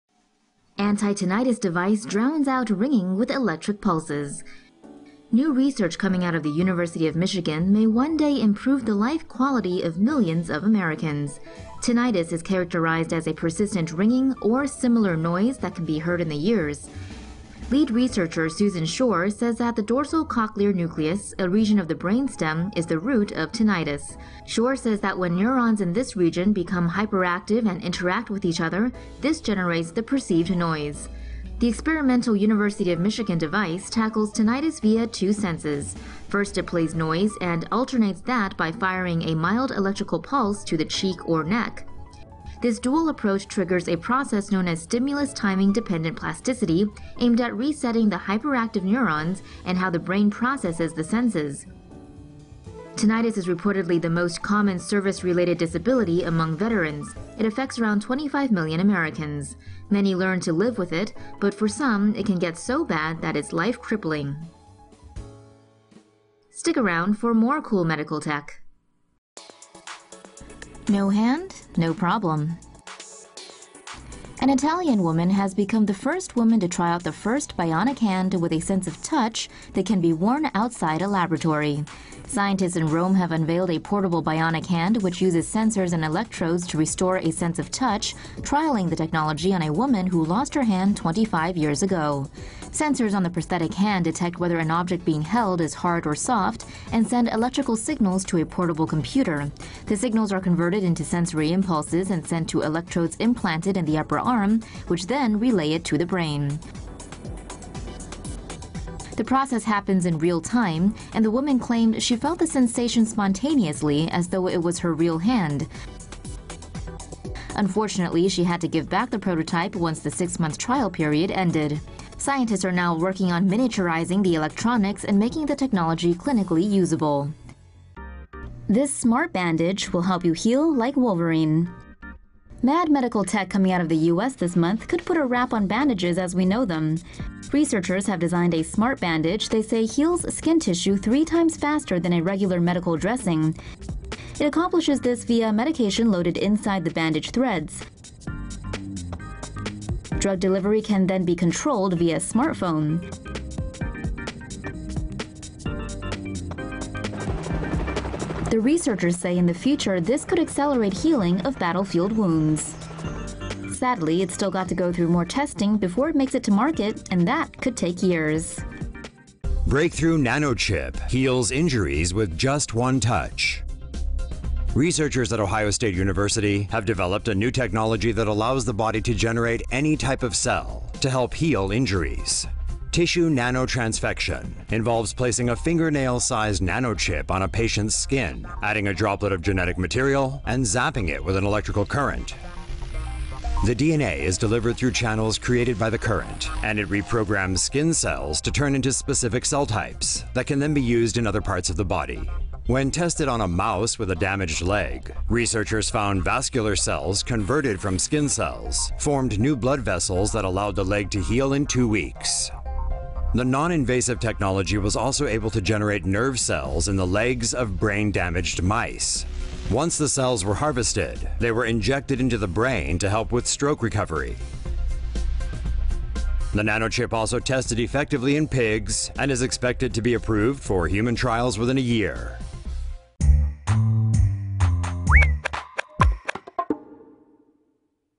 Tinnitus research: New electric device improves life of tinnitus patients - TomoNews
ANN ARBOR, MICHIGAN — A new device may hold the key to silencing the persistent ringing sufferers of tinnitus often hear.